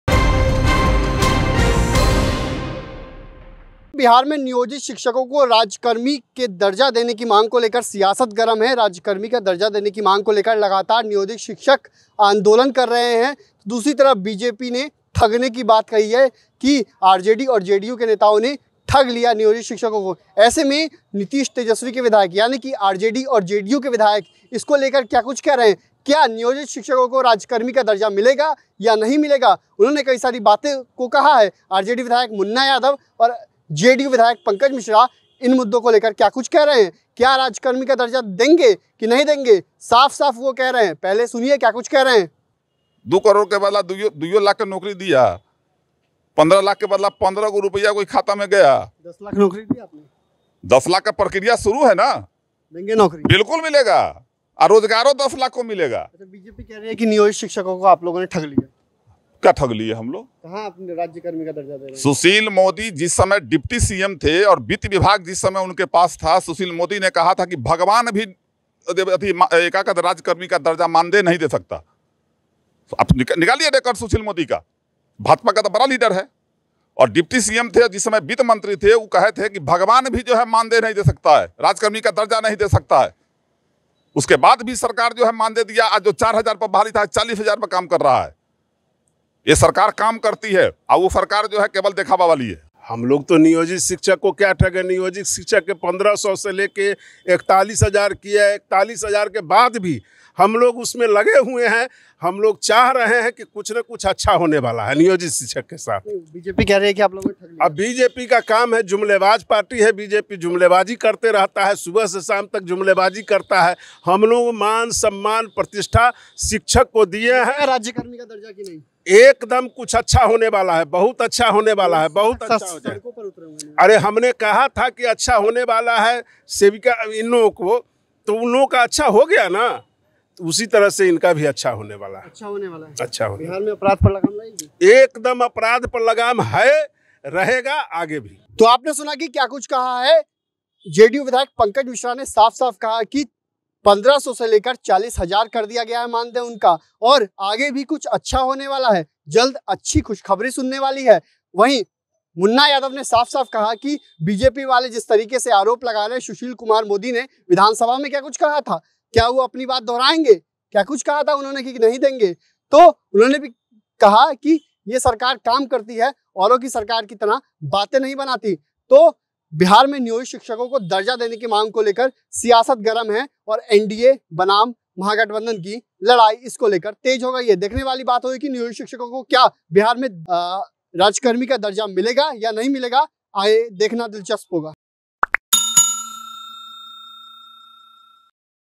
Niyojit Teachers को लेकर RJD के Munna Yadav और JDU के Pankaj Mishra ने दिया बड़ा बयान, राज्यकर्मी तो
देखें वीडियो, नियोजित शिक्षकों को कब मिलेगा राज्यकर्मी का दर्जा? नीतीश कुमार और तेजस्वी यादव के विधायकों ने सब बता दिया...